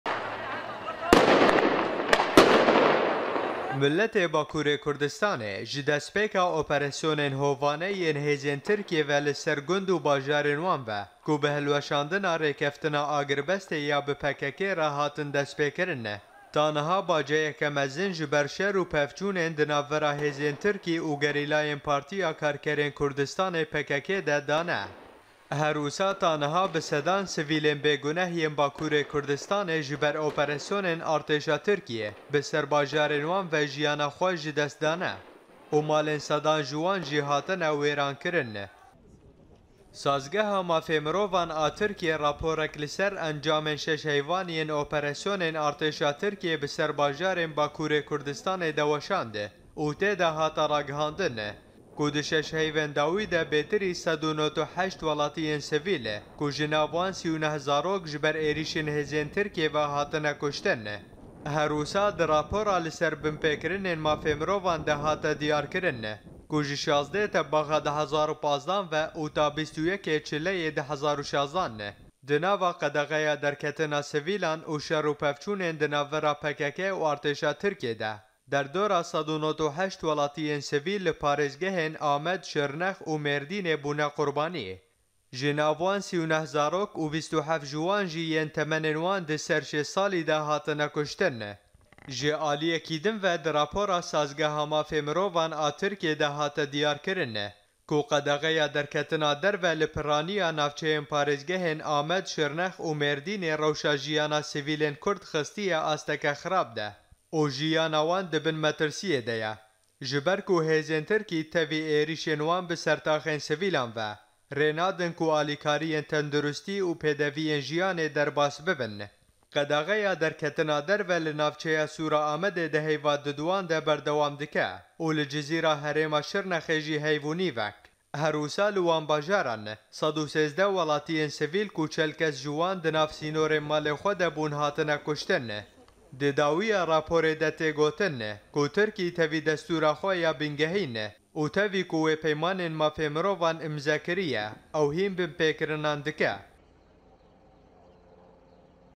Li Bakûr di 6 heyvan de 198 sivîl bûne qurbanî
Li Bakûrê Kurdistanê .. sazgeha mafê mirovan a Tirkiyê ragihand ku di 6 heyvên bûrî de bêtirî 198 welatiyên Kurd ku di nav wan de 39 zarok ji ber êrîşên û topên hêzên Tirkiyê jiyana xwe ji dest dane